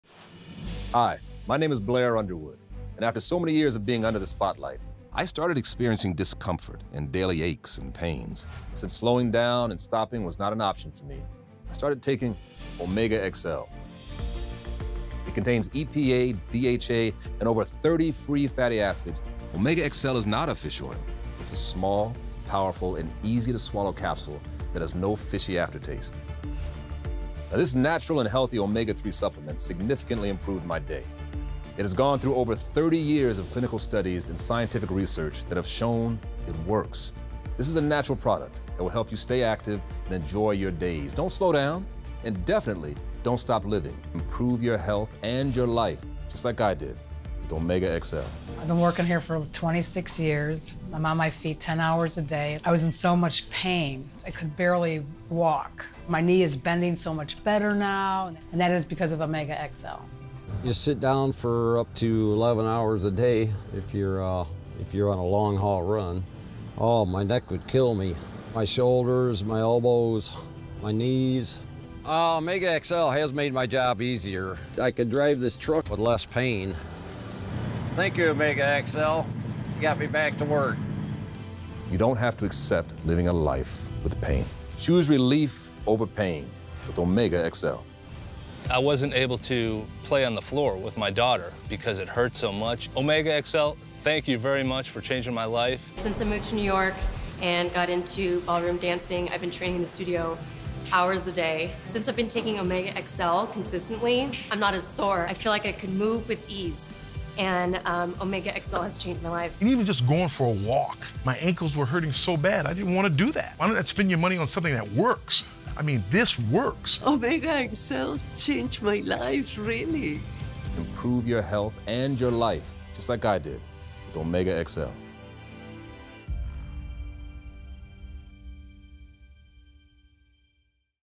IMPROVE YOUR HEALTH LIKE BLAIR UNDERWOOD WITH OMEGAXL
Actor, TV Host, and Director Blair Underwood would experience discomfort, daily aches, and pains but since slowing or stopping was not an option for him, he started taking OmegaXL® . OmegaXL®  has been shown in clinical research to support joint health and mobility.  This unique omega-3 complex contains 30 healthy fatty acids, including omega-3 DHA and EPA, and contains up to 22 times more omega-3 Free Fatty Acids than regular fish oil. Improve your health and your life, just like Blair Underwood did, with  OmegaXL®